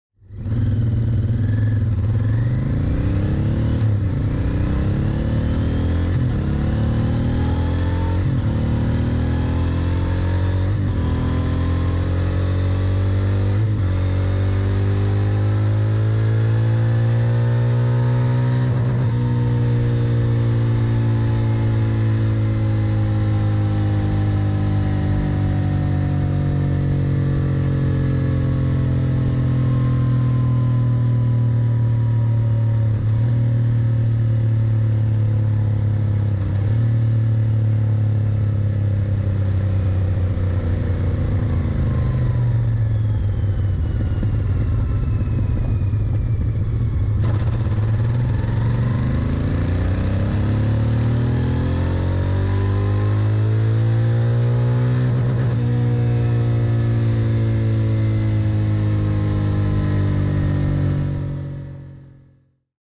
2019 Royal Enfield Continental GT 650 Dyno
Cycle World put the 2019 Royal Enfield Continental GT 650 on its dyno. How much horsepower and torque does the new parallel twin from the Indian manufacturer produce? Watch to find out.